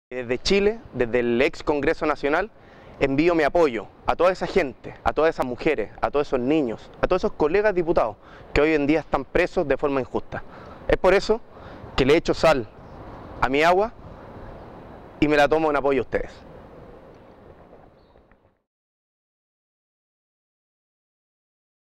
Diputado Felipe de Mussy entrega mensaje solidario a presos políticos palestinos en huelga de hambre
El Diputado Felipe de Mussy se une a SaltWaterChallenge para brindarle apoyo a los presos políticos palestinos en huelga de hambre. ¿Y tú, que esperas para unirte a este digna campaña? ¡Puedes enviarnos tu video con un mensaje solidario!